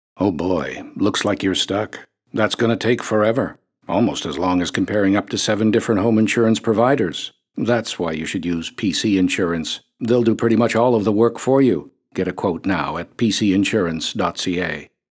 PC Home Insurance: Stuck Zipper (short)
PC Insurance presents: Things that are still better than shopping around for insurance.

Fixing a stuck zipper may take a while, but not as long as it takes to compare upto 7 insurance providers on your own. Let PC Insurance do the work for you.

Get a home insurance quote today.